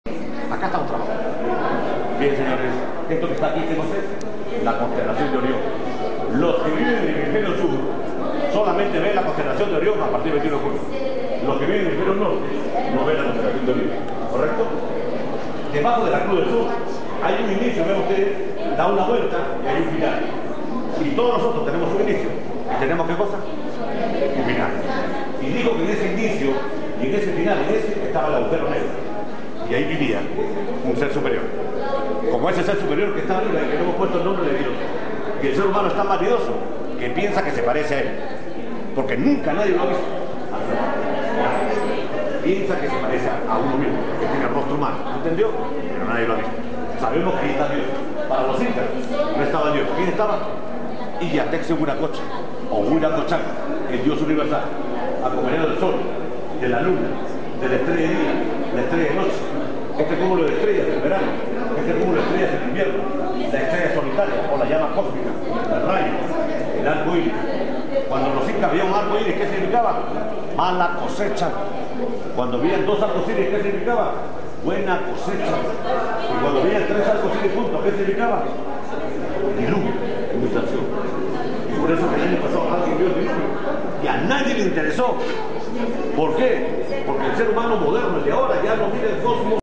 LAS ESTRELLAS SEGÚN LOS INCAS, CUSCO QORIKANCHA
Explicación de Jorge sobre el significado de las estrellas segun los Incas. Lugares turisticos en Peru: Qorikancha Cusco